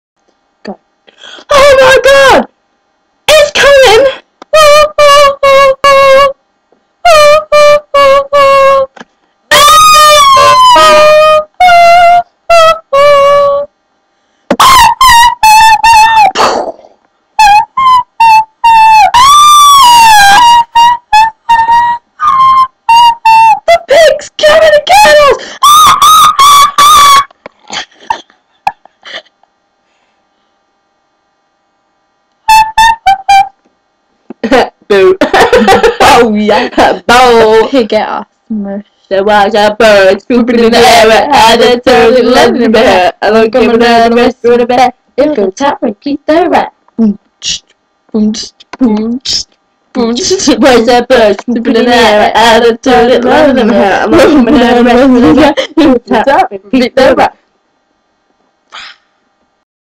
sam and jodi ( giant pig)
me and jodi basically messing around with the camera